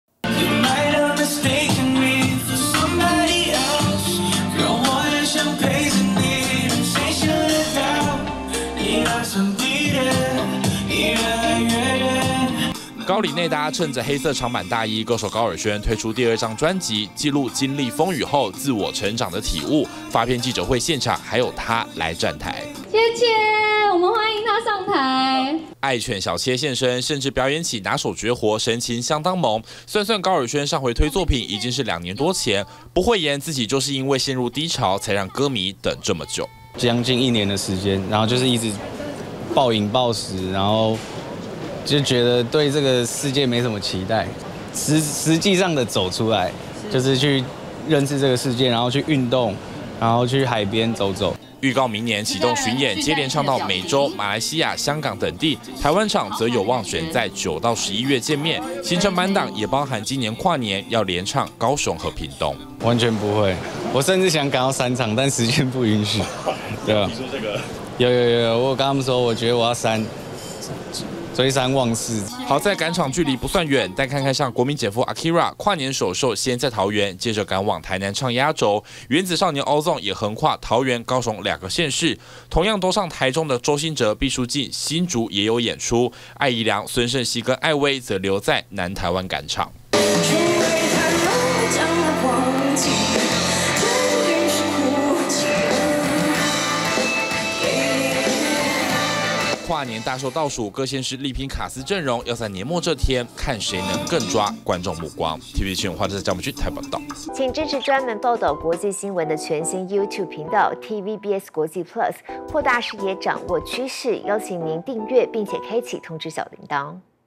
高爾宣迎新輯認「過去一年低潮」曝在台辦個唱時程「跨年趕2攤」｜TVBS娛樂頭條
跨年活動倒數，各縣市祭出超強卡司陣容，其中不少歌手當晚得跨縣市趕著演出；像是高爾軒就連唱高雄及屏東兩場，他表示近期剛好發新專輯，自己甚至跟公司提過，希望能一晚連唱3到4場，畢竟很期待能有大型演出的機會。